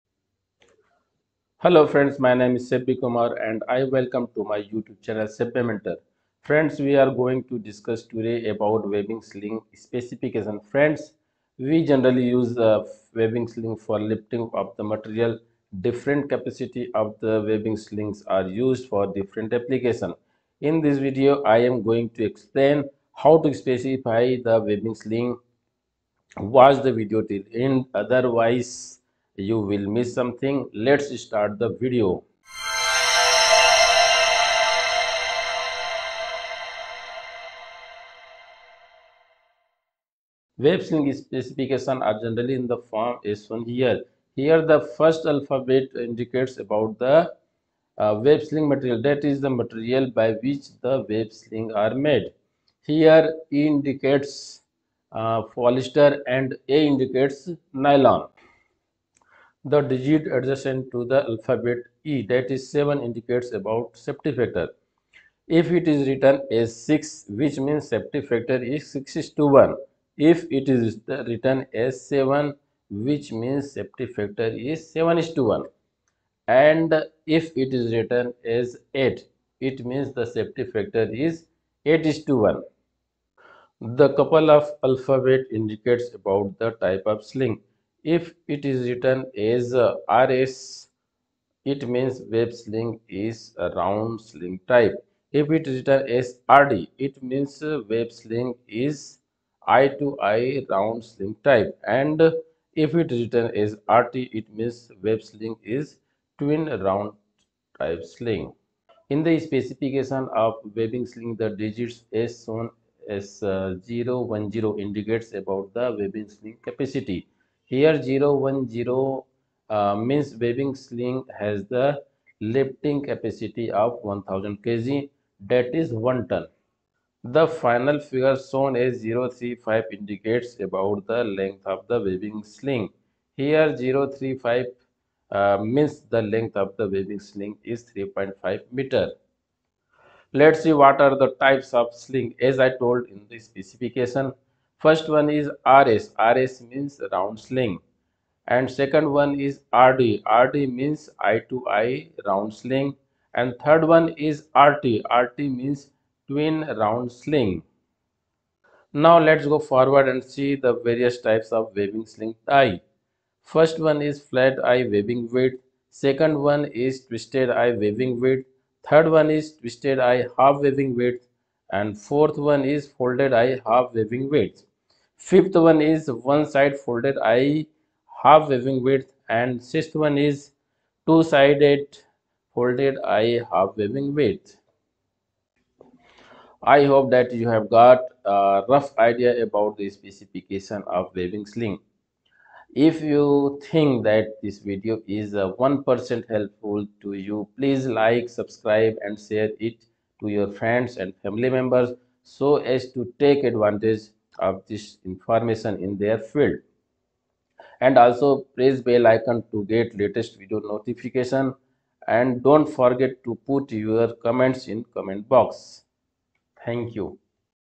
Web sling specification | Lifting belt specification | How to specify web sling & lifting belt
In this video I have explained about the specification of the web sling or Lifting belt. Web sling or lifting belts are used to lift the material in the industry . Different capacity web slings are used for different application. There are various types of web slings or lifting belt , all about that I have explained in this video.
I hope you will find the video very informative.

My previous video links are appended here .

Related SEARCH .
Lifting belt,
Web sling,
Webbing sling,
Webbing belt specification